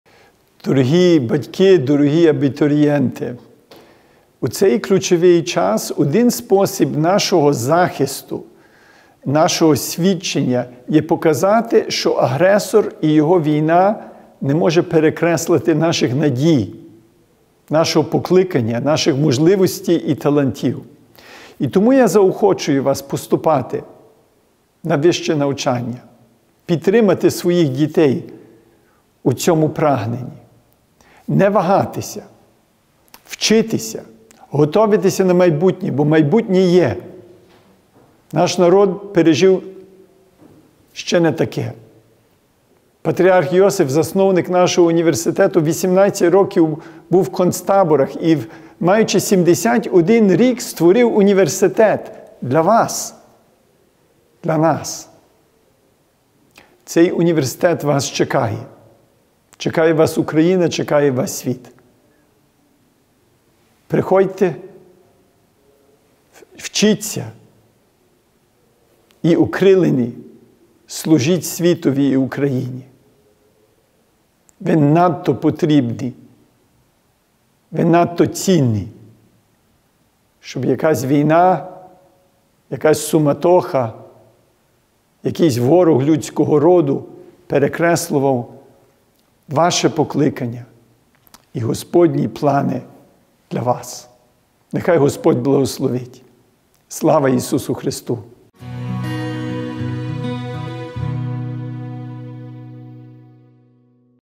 Приходьте в УКУ, вчіться, служіть світові та Україні!
Владика Борис Ґудзяк, президент УКУ, митрополит Філадельфійської архиєпархії в США звернувся до абітурієнтів та їхніх батьків.
«У цей ключовий час один зі способів нашого захисту, нашого свідчення — показати, що агресор і його війна не може перекреслити наших надій, покликання, можливостей і талантів. Я заохочую вас вступати на вищі студії, а батьків заохочую підтримати своїх дітей у цьому прагненні. Не вагатися. Готуватися на майбутнє, бо майбутнє є!», — каже владика Борис.
Дивіться повне звернення владики у відео.